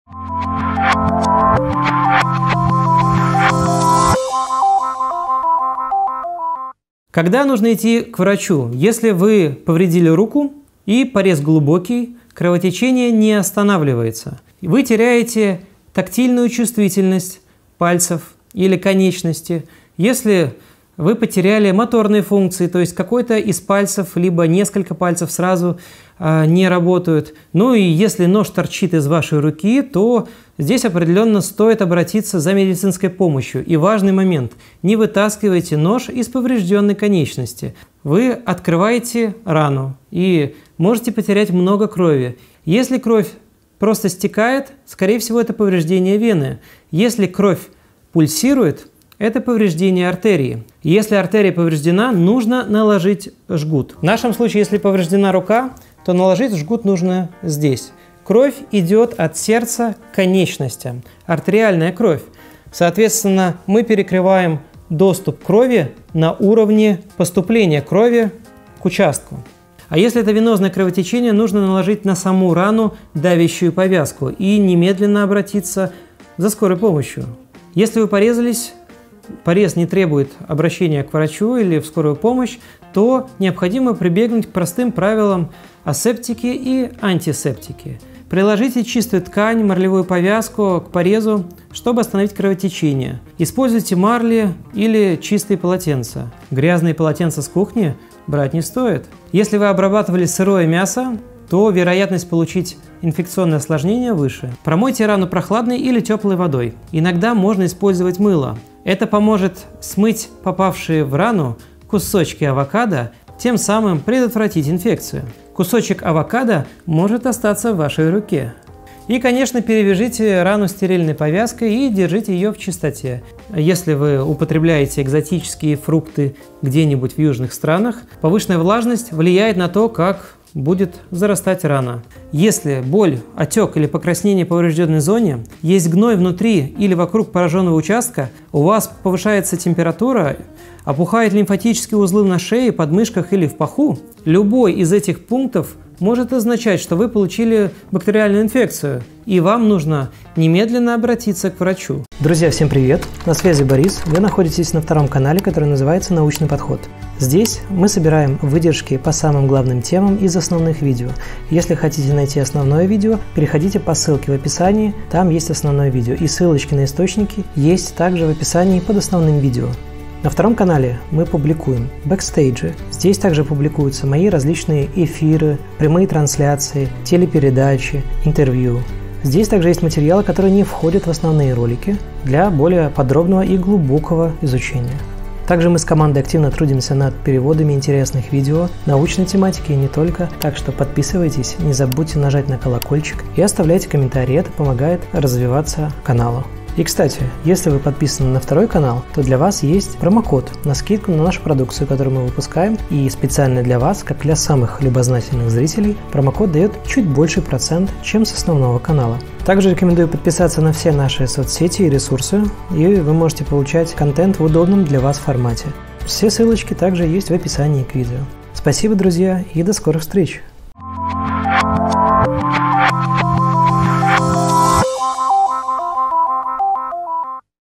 Нужны ли ВРАЧИ?
Травмы кухонным ножом — не редкость. Но в каких же случаях можно обойтись без похода к врачу, а в каких случаях врач НЕОБХОДИМ? Рассмотрим разные виды потери крови и разберёмся, что с этим делать.